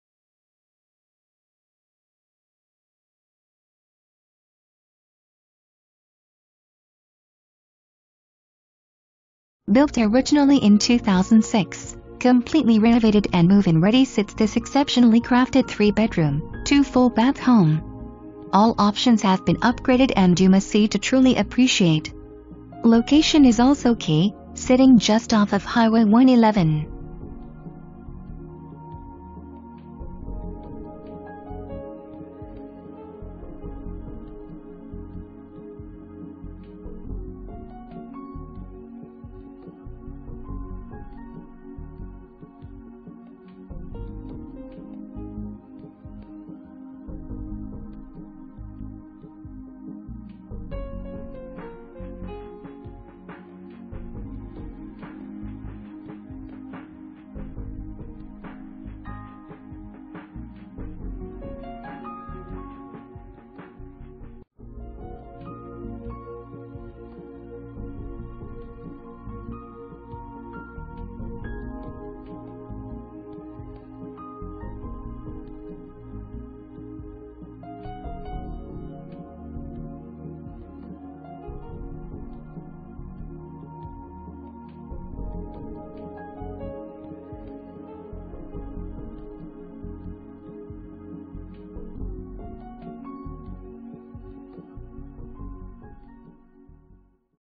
255 Derby Ln.
255 Derby Ln.

Built originally in 2006, completely renovated and move in ready sits this exceptionally crafted 3 bedroom, 2 full bath home. All options have been upgraded and you must see to truly appreciate! Location is also key, sitting just off of Hwy 111.

Presented By:
Christopher Cunningham Realtor, American Way Real Estate